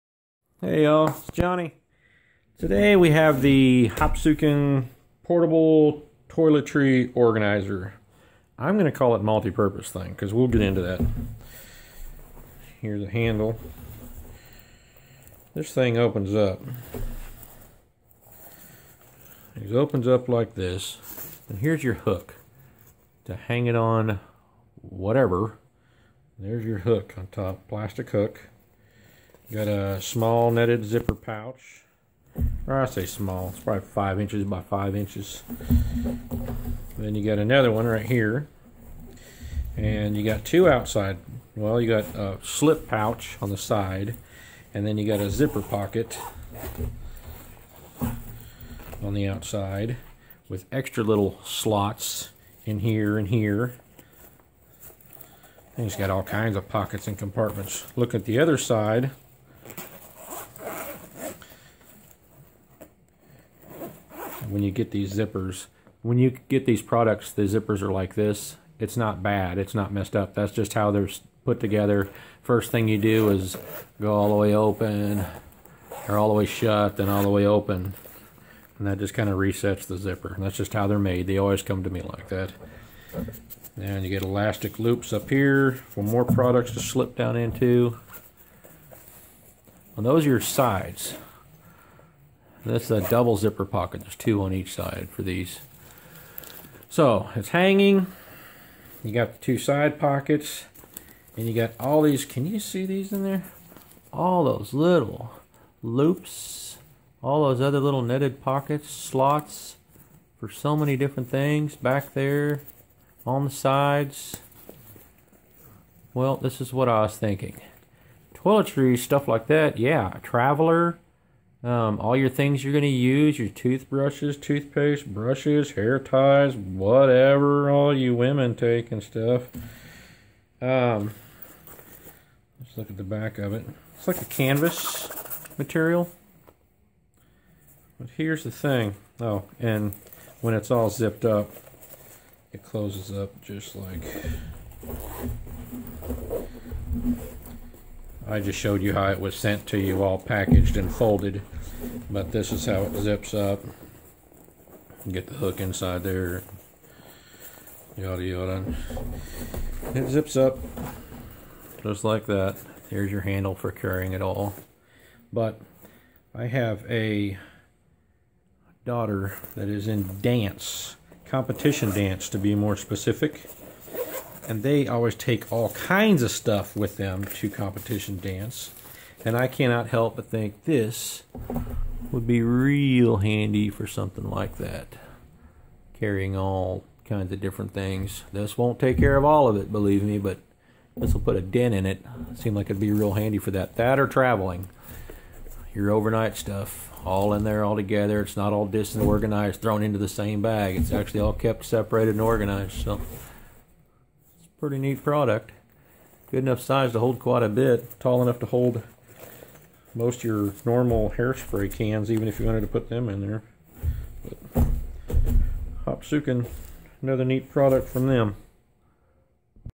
Hopsooken Travel toiletry bag
I bought this for the wife and daughter to use during dance competitions. There's ample room for just about anything you need. It is also great as a travel toiletry bag for which it's designed.